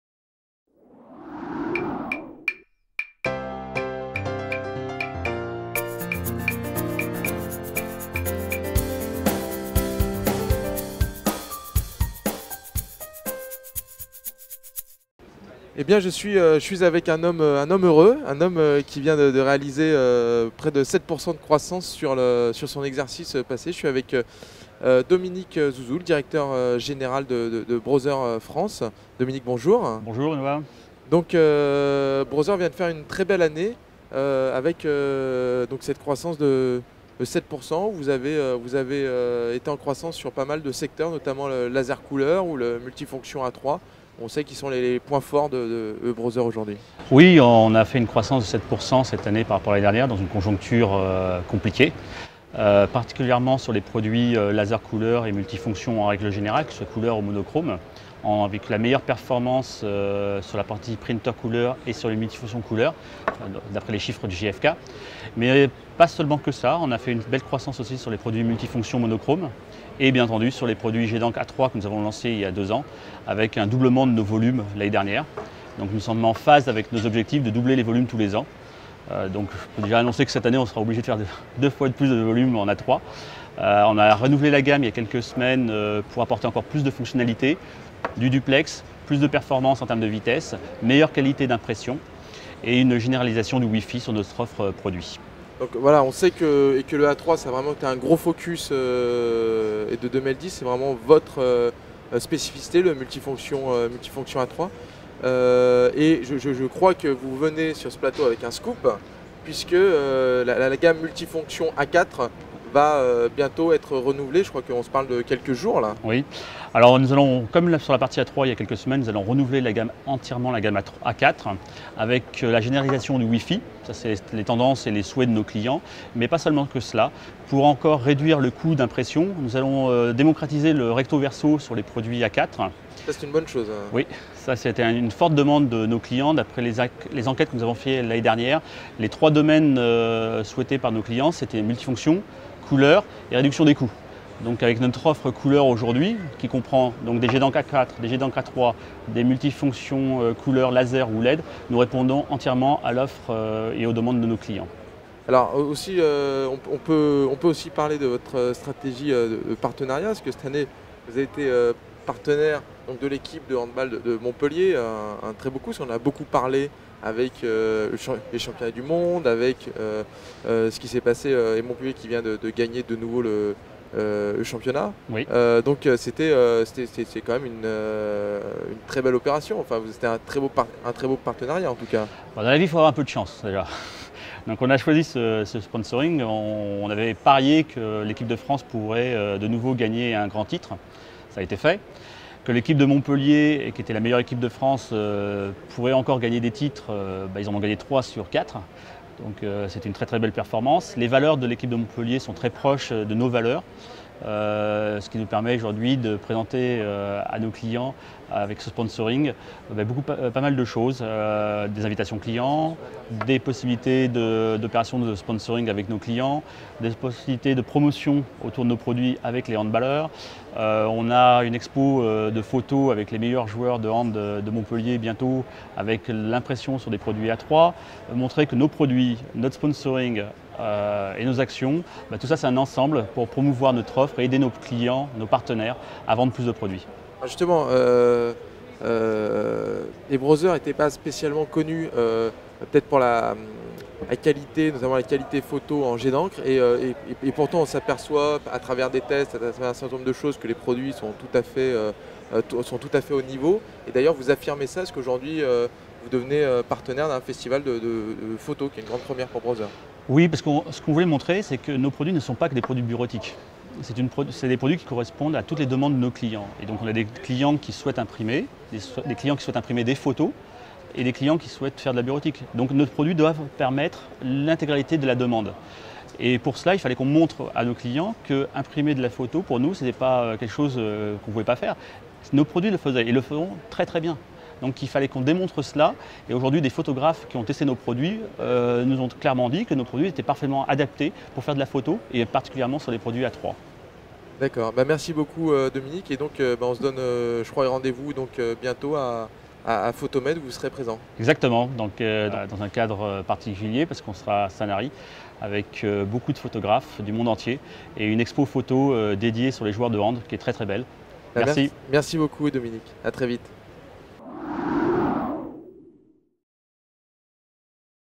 Dominique Zouzou fait un point sur la belle croissance de la marque
Au MEDPI 2011, sur le plateau TV de Photobusiness.fr, Dominique Zouzou revient sur la belle progression de Brother sur le marché de l'impression cette année et annonce le renouvellement de la gamme multifonction A4 avec notamment une généralisation du WiFi et une démocratisation du recto-verso.